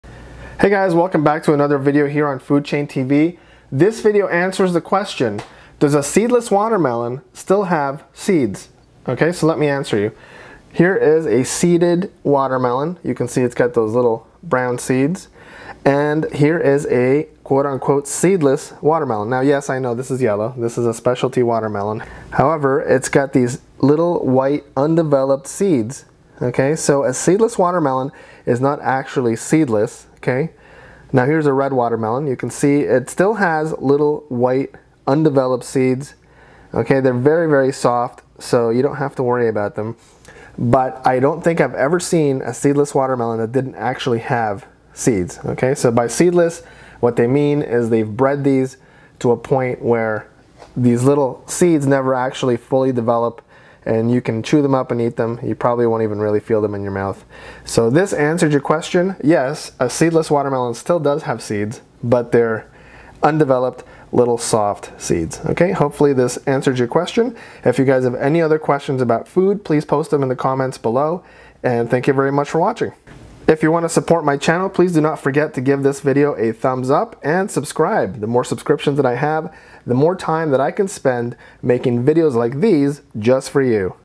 Are seedless watermelon actually seedless? Watch this video and find out!
Seedless watermelon are not actually seedless. Seedless watermelon have been bred to keep their seeds from maturing into hard brown seeds, but they still do have soft, pale, undeveloped seeds which are soft and tasteless.

If you guys have any other food questions, please let me know.